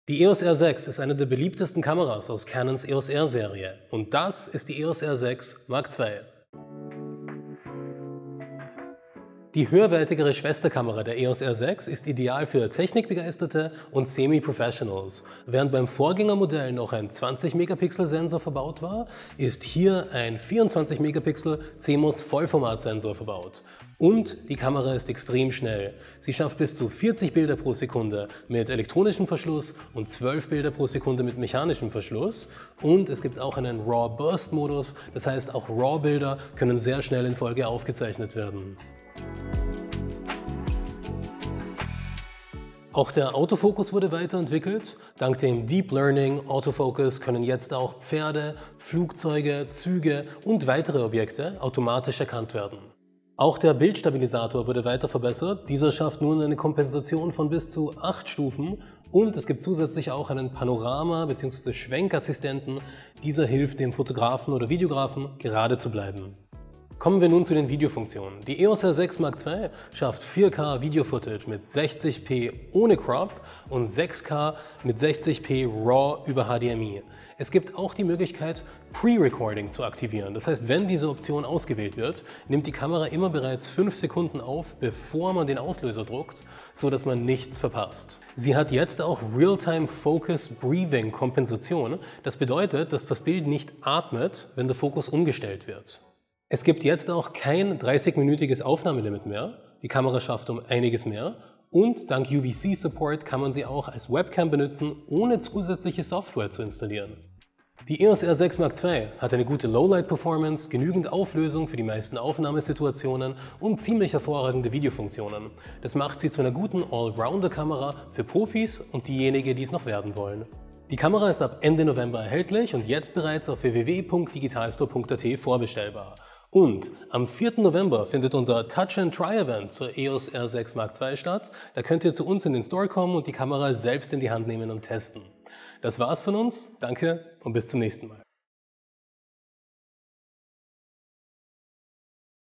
Neu: Canon EOS R6 Mark II (Alle Infos im Hands-On Video!)
Entdecke die neue Canon EOS R6 Mark II:
Alle Infos zum neuesten Release aus dem Hause Canon findest du in diesem exklusiven Hands-On Video mit Joel Abraham Shroff! Am 4.11 hast du die Chance die Kamera selbst in die Hand zu nehmen und zu testen - bei dem ersten Canon VIP Table zur EOS R6 Mark II im Digitalstore Vienna. Wir freuen uns auf deinen Besuch!